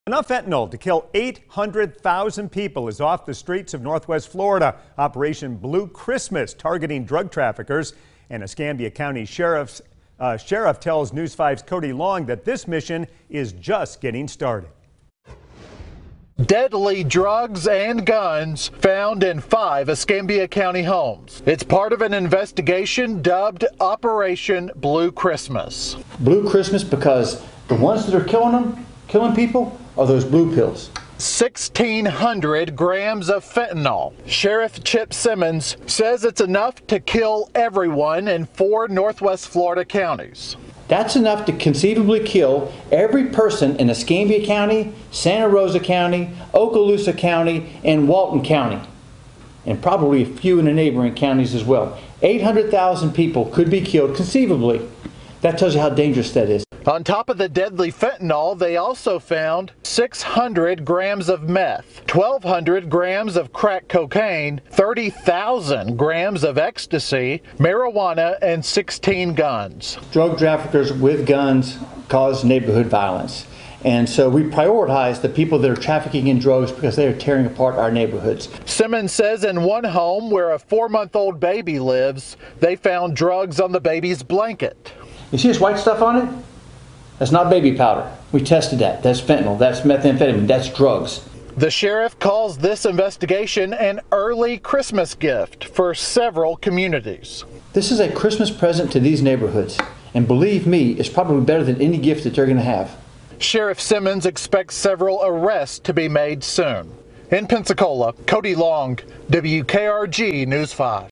1,600 grams of fentanyl, 16 guns, other narcotics seized in ‘Operation Blue Christmas’: Escambia Co.
The Escambia County Sheriff’s Office Narcotics Unit has served five separate search warrants over the last week as part of “Operation Blue Christmas.”